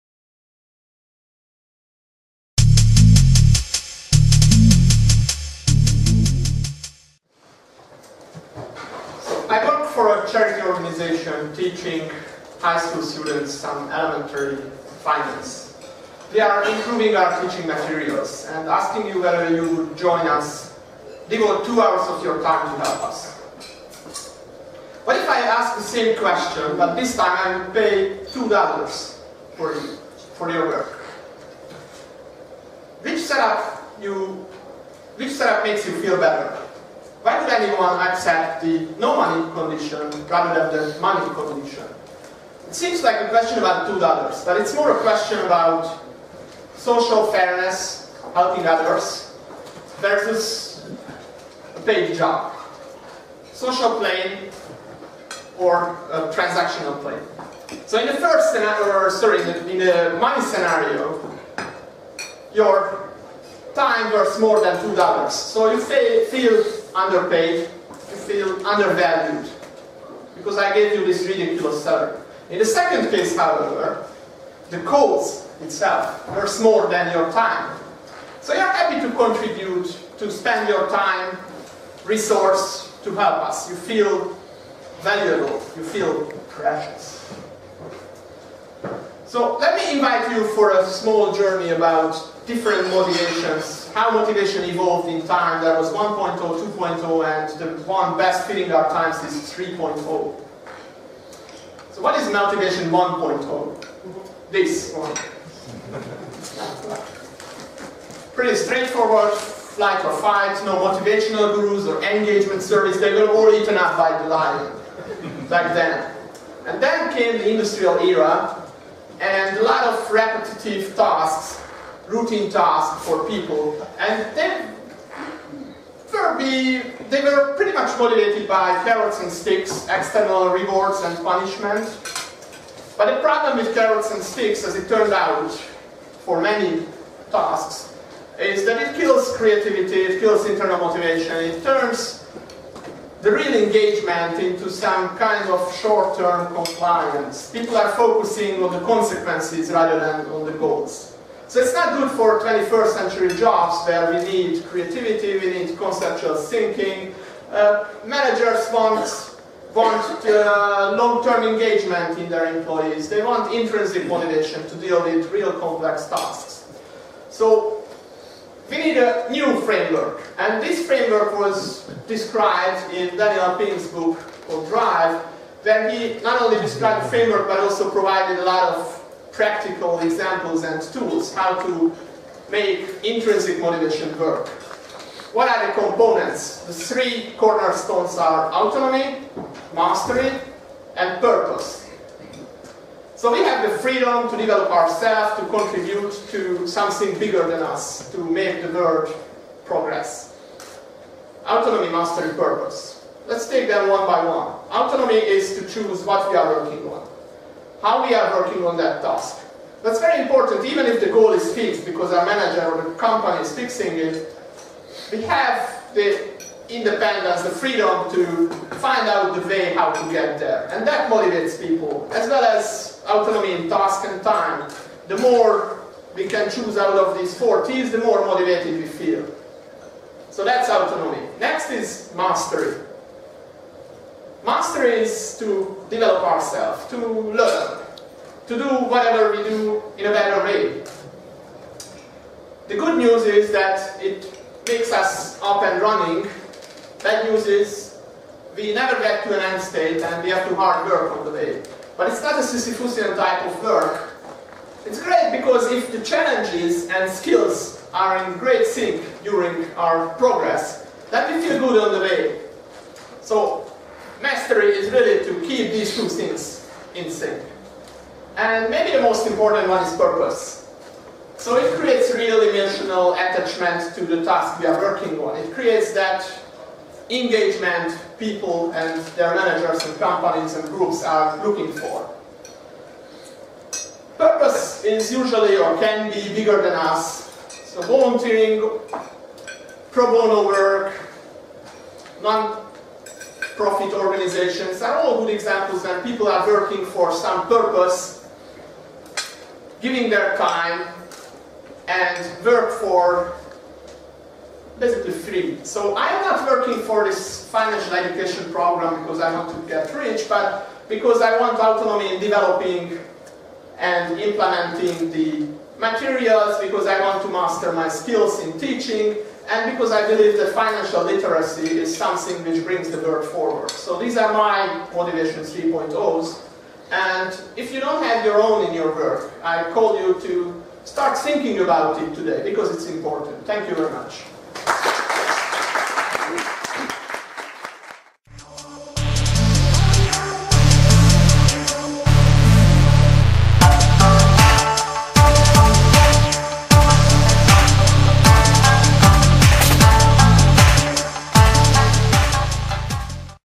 Gábor Salamon - "Motivation 3.0"
What motivates people to do 21st century jobs which need creativity, innovation and conceptual thinking? Do rewards and punishments make them working better?

Presented at Ignite Budapest #5, Brody Studios, Budapest, Hungary, May 17, 2017.